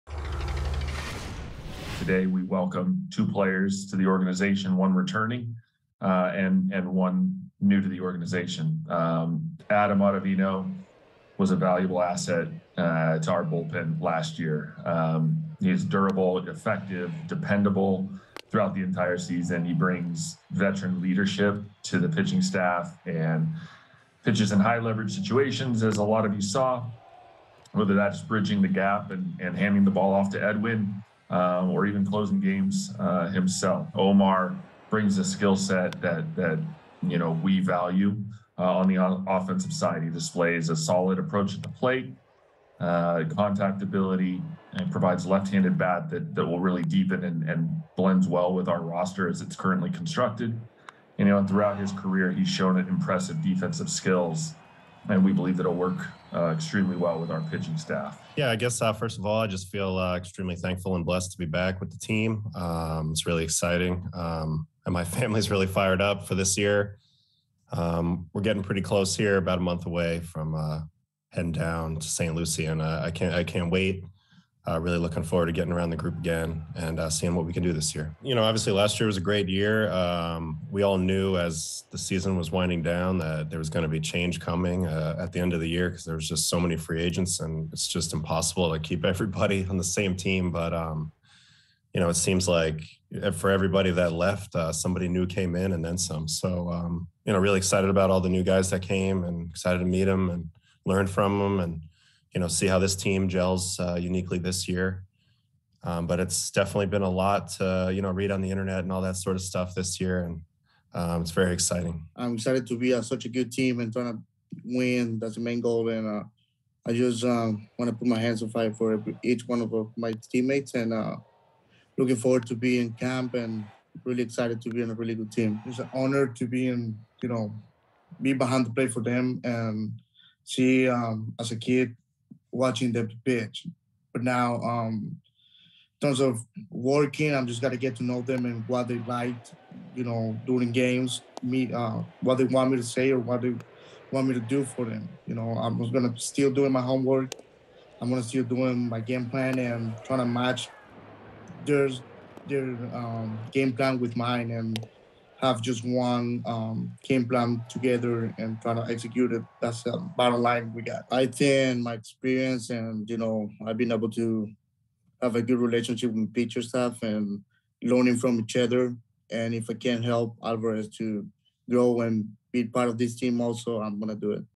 Mets Introduce Two Key Offseason Acquisitions
General manager Billy Eppler introduces catcher Omar Narváez and right-handed pitcher Adam Ottavino to the media.

Category
Sports